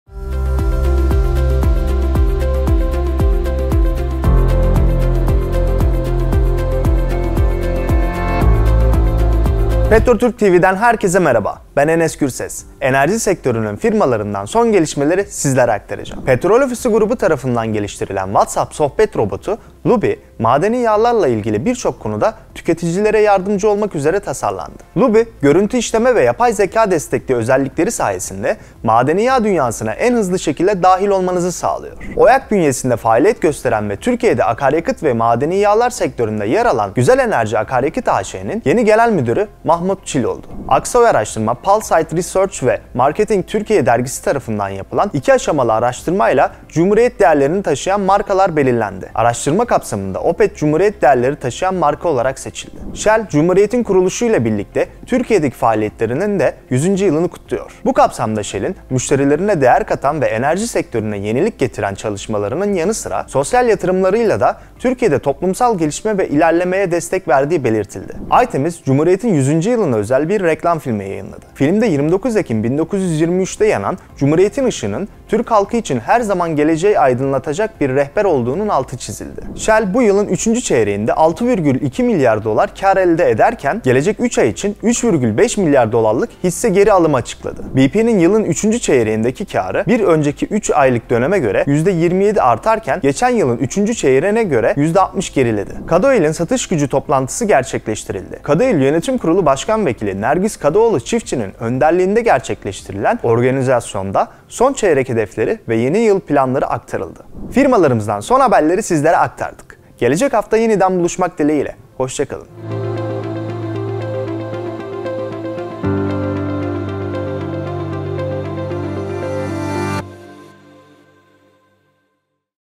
GüzelEnerji Akaryakıt A.Ş.’nin yeni Genel Müdürü Mahmut Çil oldu | ŞİRKETLERDEN HABERLER
Petroturk TV Muhabiri Enes Gürses, enerji şirketlerinde yaşanan gelişmeleri sizler için derledi

Petrol Ofisi WhatsApp sohbet robotu ‘Lubi’yi geliştirdi

GüzelEnerji Akaryakıt A.Ş.’nin yeni Genel Müdürü Mahmut Çil oldu

‘Cumhuriyet Değerlerini Taşıyan Marka’ Opet

‘Shell, 100 yıldır gururla Türkiye’nin yanında’

‘Bi’ kere Cumhuriyet, Hep Cumhuriyet’

Shell, 3. çeyrekte 6,2 milyar dolar kar elde etti

bp, üçüncü çeyrekte 3,3 milyar dolar kar sağladı

Kadoil Satış Gücü Toplantısı’nı gerçekleştirdi

Petroturk TV
Enerjinin Haber Merkezi